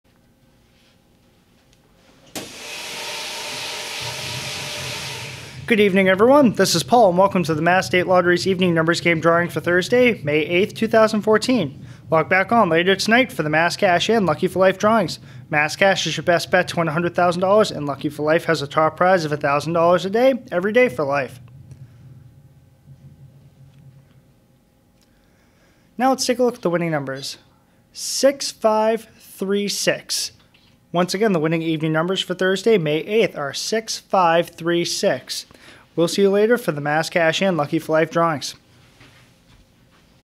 Evening Numbers Game Drawing: Thursday, May 8, 2014
The official winning numbers in the Mass. State Lottery's Evening Numbers Game drawing for Thursday, May 8, 2014 are: 6-5-3-6.